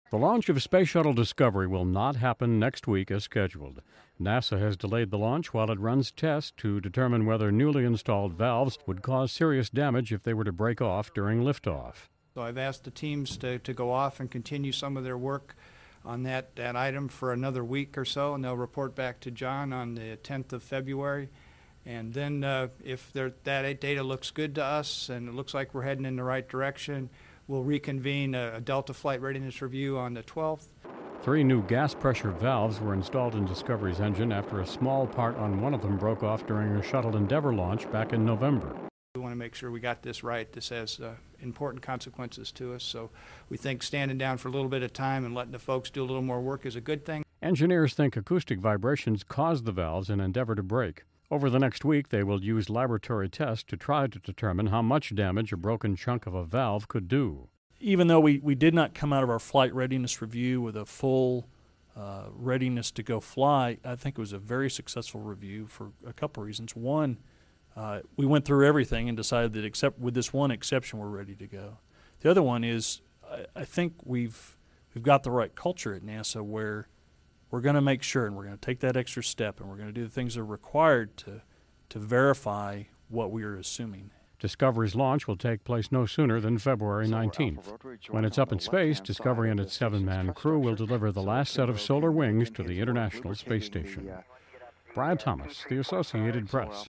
Launch of Space Shuttle Discovery Delayed
The launch of the space shuttle Discovery will not happen next week, as scheduled.  The space agency delayed the launch while it runs tests to determine whether newly installed valves would cause serious damage if they broke during lift-off. (Feb. 4)